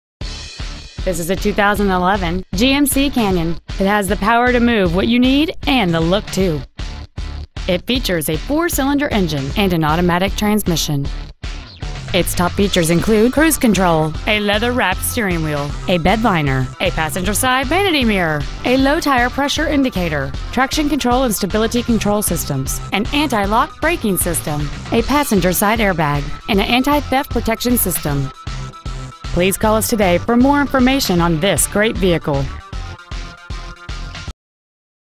2011 GMC Canyon IL
We are proud to present this 2011 GMC Canyon .

 Year : 2011
 Make : GMC
 Model : Canyon
 Engine : 2.9L I4
 Trans . : Automatic
 Exterior : Silver

 Interior : Black
 Grossinger Autoplex
 6900 N. McCormick Blvd
  , IL 60712